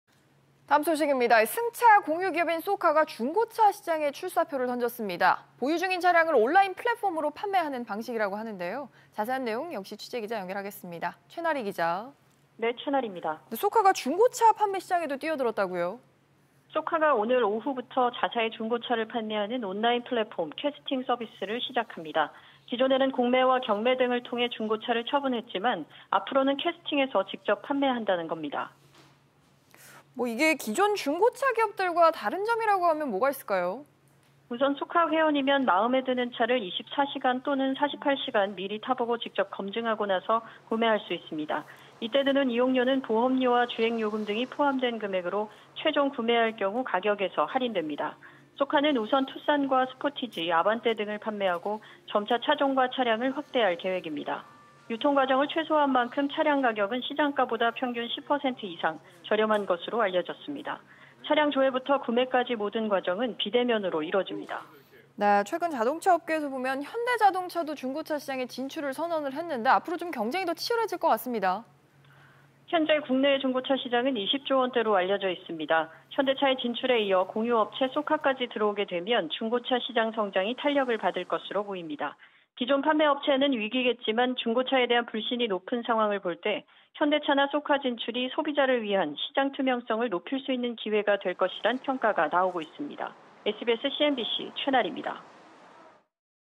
쏘카, 온라인 중고차 플랫폼 출시...“평균 10% 이상 저렴”
[앵커]

승차 공유업체 쏘카가 중고차 시장에 진출합니다.

보유 중인 차량을 온라인 플랫폼으로 판매하는 방식인데요.

자세한 내용 최나리 기자 연결해 알아보겠습니다.

쏘카가 중고차 판매 시장에 뛰어들었다고요?